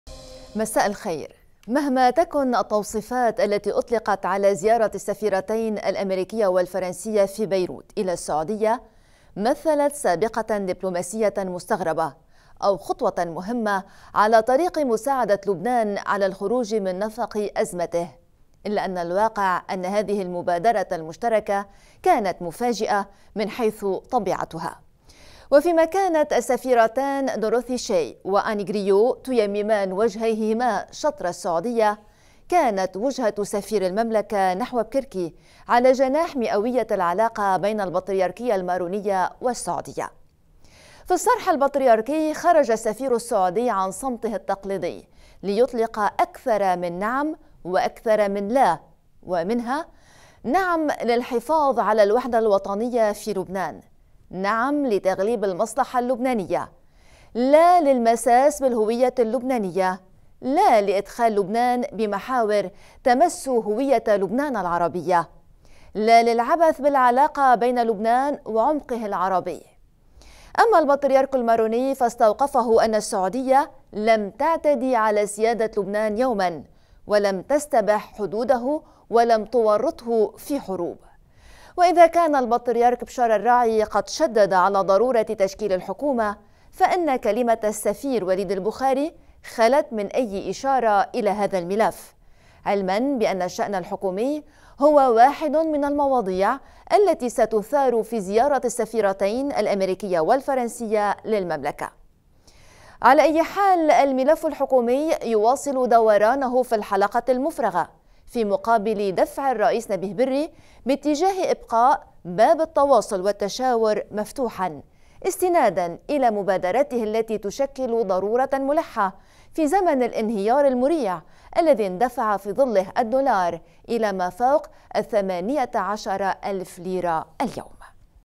مقدمة النشرة 08-07-2021 - مهما تكن التوصيفات التي أُطلقت على زيارة السفيرتين الأميركية والفرنسية
مهما تكن التوصيفات التي اُطلقت على زيارة السفيرتين الأميركية والفرنسية في بيروت إلى السعودية/  مثـّلت سابقة دبلوماسيةً مستغَربة/ أو خطوة مهمة على طريق مساعدة لبنان على الخروج من نفق أزمته/ إلا أن الواقع أن هذه المبادرة المشتركة كانت مفاجئة من حيث طبيعتُها//.
وفيما كانت السفيرتان (دوروثي شيا) و(آنّ غريو) تُيممان وجهيهما شطر السعودية/ كانت وجهةُ سفير المملكة نحو بكركي على جناح مئوية العلاقة بين البطريركية المارونية والسعودية//.
في الصرح البطريركي/ خرج السفير السعودي عن صمته التقليدي ليطلق أكثر من (نعم) وأكثر من (لا) ومنها://.
- نعم للحفاظ على الوحدة الوطنية في لبنان//.
- نعم لتغليب المصلحة اللبنانية//.
* لا للمساس بالهوية اللبنانية//.
* لا لإدخال لبنان بمحاور تَمَس هوية لبنان العربية//.
* لا للعبث بالعلاقة بين لبنان وعمقه العربي//.
اما البطريرك الماروني فاستوقفه ان السعودية لم تَعتدِ على سيادة لبنان يومًا/ ولم تستبح حدوده/ ولم تورِّطْه في حروب//.
وإذا كان البطريرك بشارة الراعي قد شدد على ضرورة تشكيل الحكومة/ فإن كلمة السفير وليد البخاري خَلَت من أي إشارة إلى هذا الملف/ علمًا بأن الشأن الحكومي هو واحد من المواضيع التي ستُثار في زيارة السفيرتين الأميركية والفرنسية للمملكة//.
على أي حال/ الملف الحكومي يواصل دورانَه في الحلقة المفرغة/ في مقابلِ دفْعِ الرئيس نبيه بري باتجاه إبقاء باب التواصل والتشاور مفتوحًا/ إستنادًا إلى مبادرته/ التي تشكل ضرورة ملحة/ في زمن الإنهيار المريع الذي اندفع في ظله الدولار إلى ما فوق ثمانية عشر ألف ليرة اليوم//.